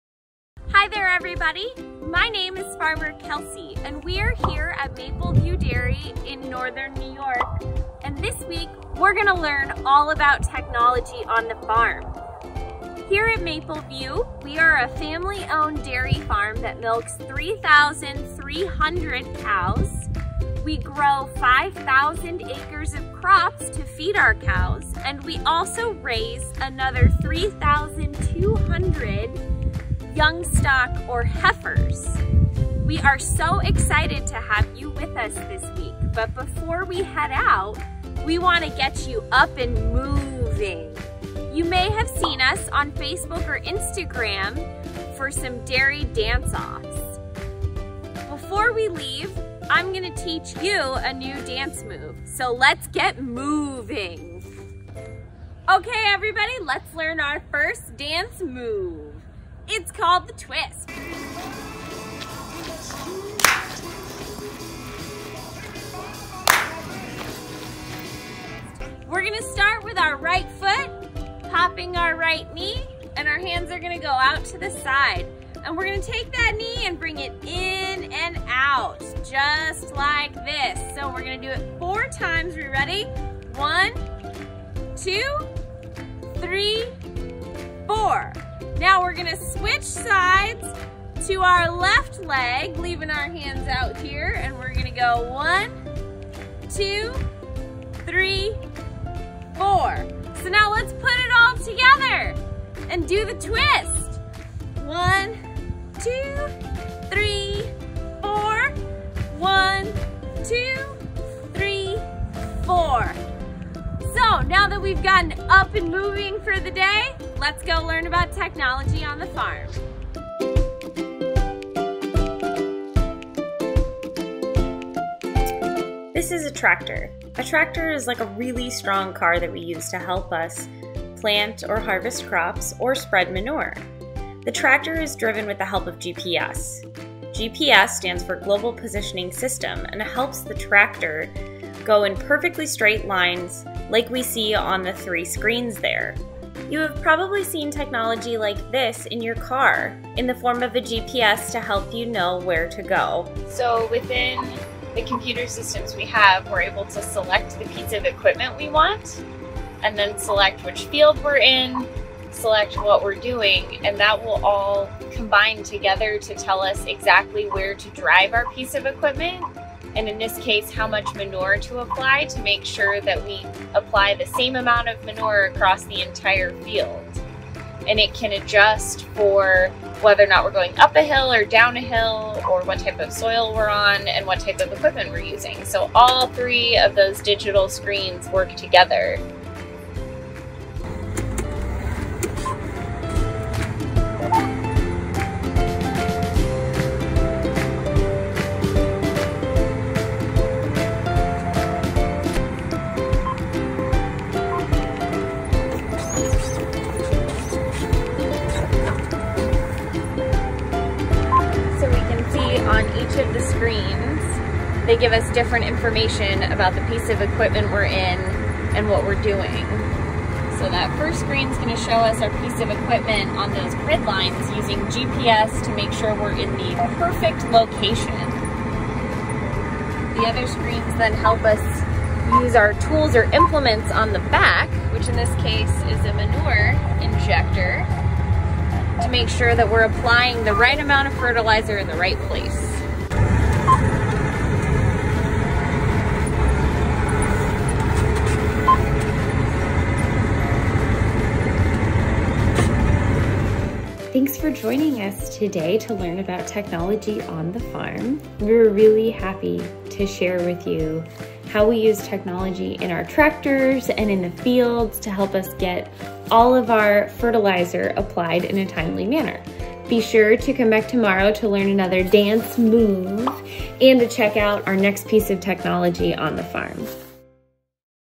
Fun on the Farm | Farmer Kelsey | GPS
Join Farmer Kelsey and learn how the technology you use to navigate on the road also helps farmers in the fields and learn a few dance mooves in the process 💃🐄!